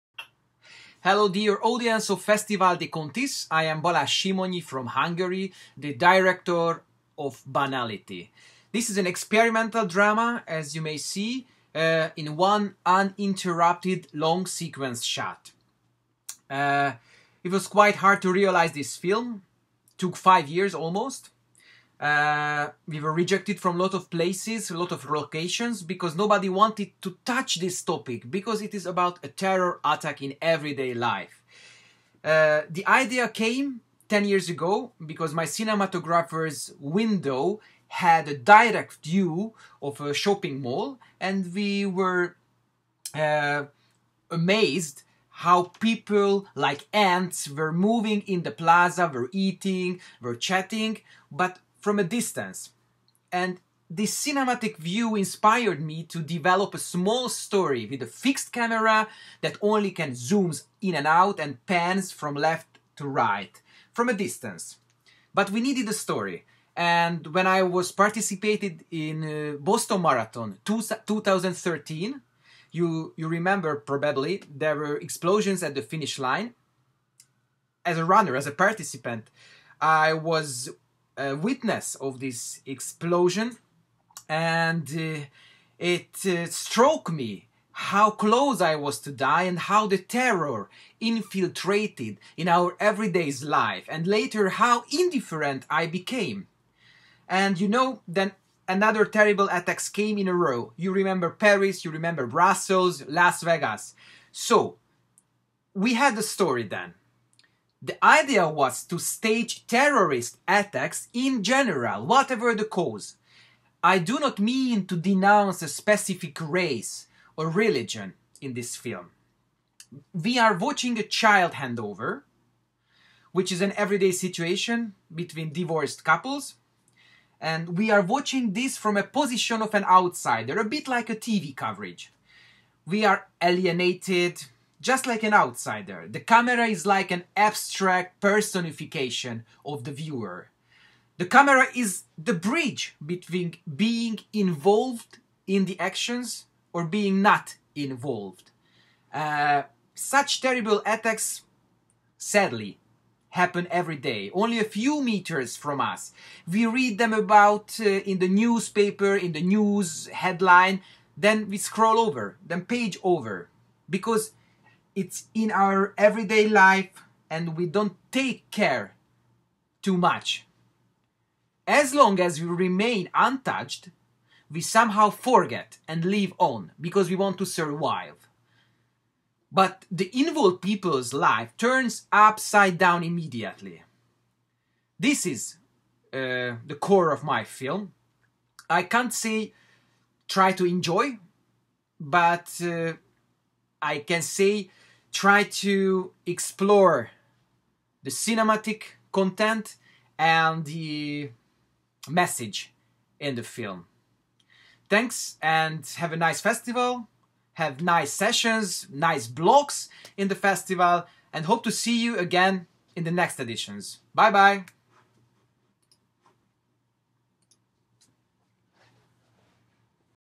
[FESTIVAL DE CONTIS 2020] - Balázs Simonyi présente "BANALITY"
Le réalisateur Balázs Simonyi vous présente son court-métrage "BANALITY" sélectionné en Compétition européenne de courts-métrages du 25e Festival de Contis (du 2 au 6 juillet 2020).